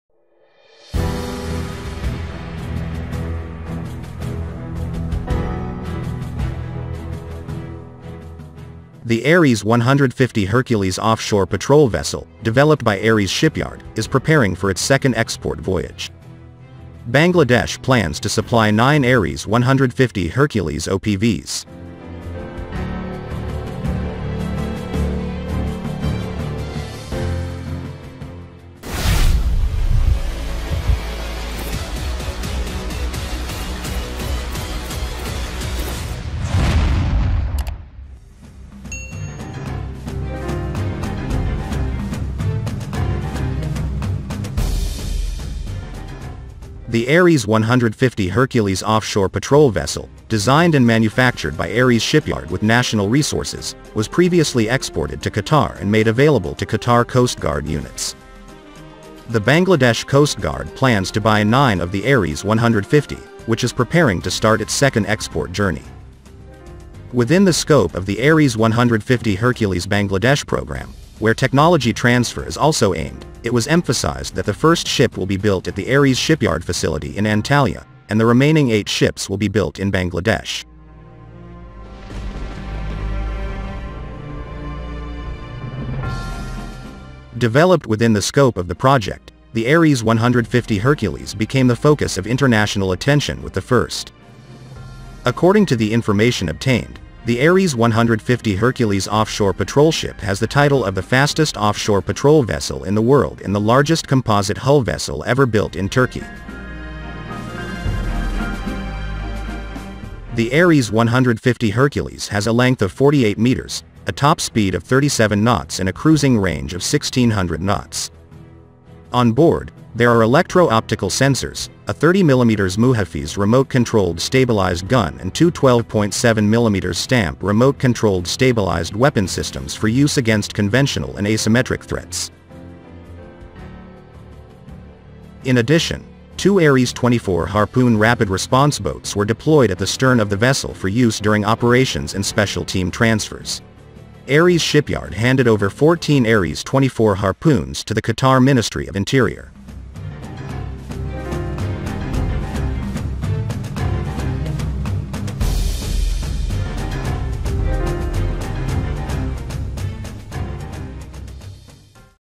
Bangladesh Plans to Supply 9 Offshore Patrol Vessel from Turkish
The Ares 150 Hercules Offshore Patrol Vessel, developed by Ares Shipyard, is preparing for its second export voyage. Bangladesh plans to supply 9 Ares 150 Hercules OPVs.